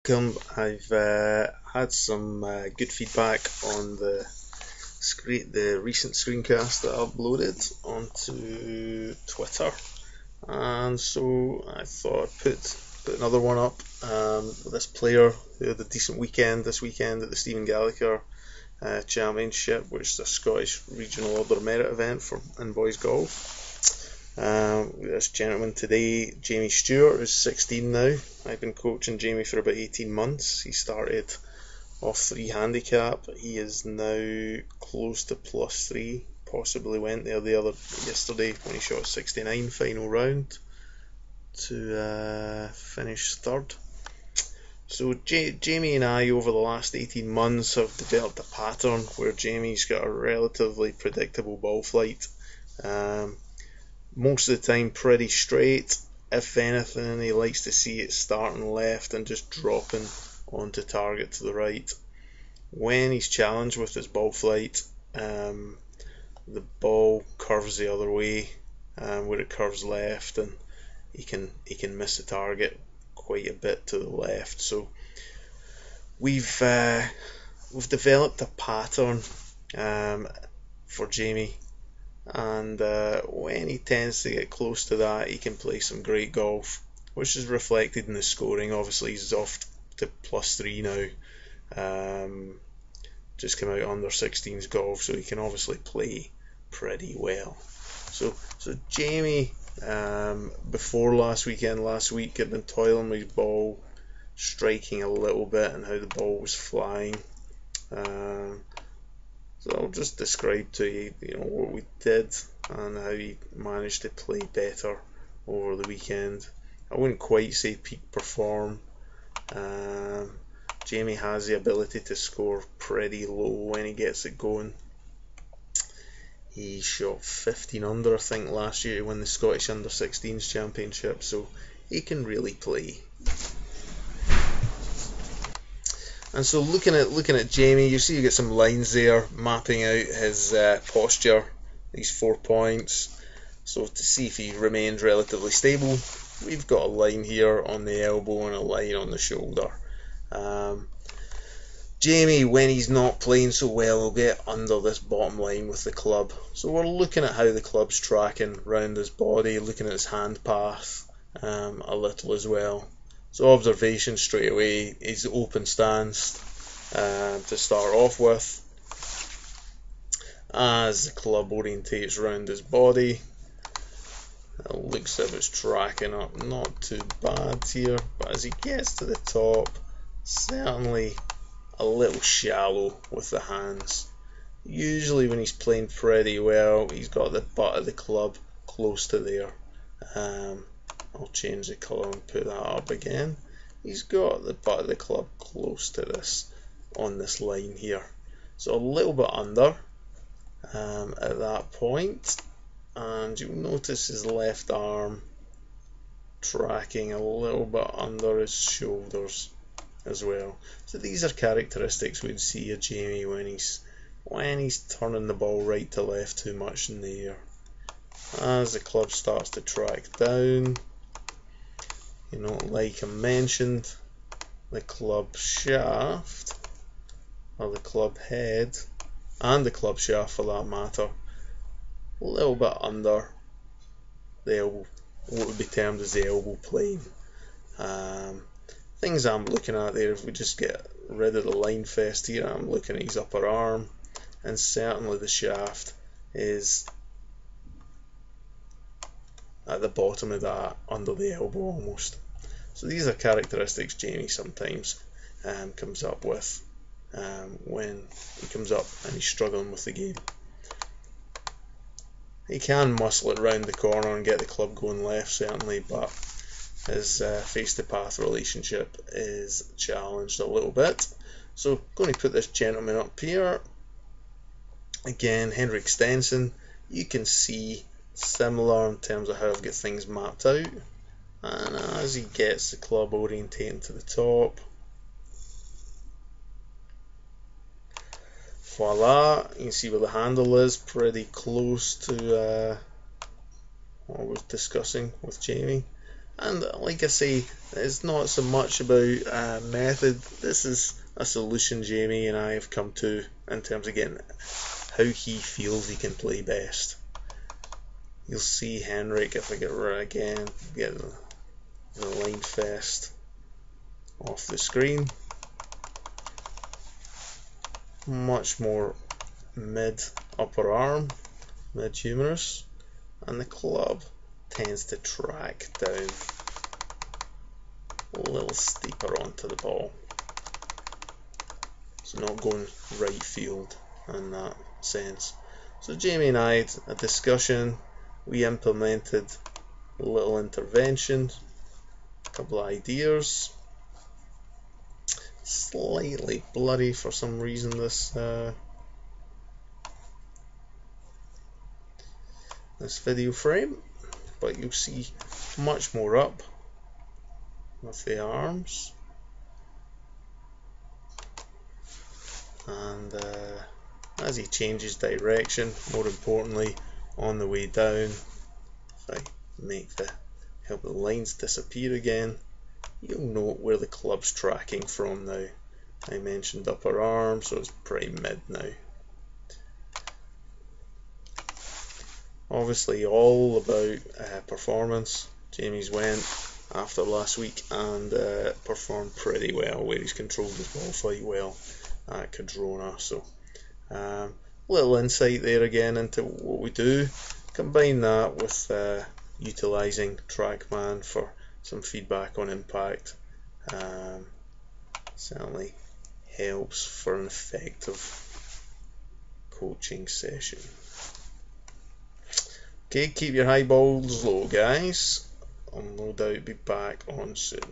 Jamie Stewart - Scottish Under 16's Champion
A short clip of technical movement adjustment made during coaching session utilising TRACKMAN software